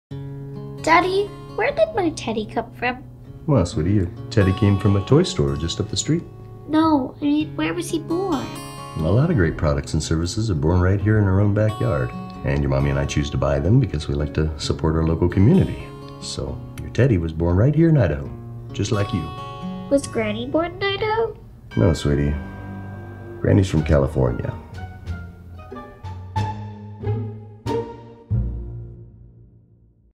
Buy Idaho - Born in Idaho
We just wrapped up a little silly PSA/Commercial for Buy Idaho. "Was Granny born in Idaho?"  Let me tell you a little something about that :)

P.S. - We love you California!

Check out Buy Idaho and all of their awesome services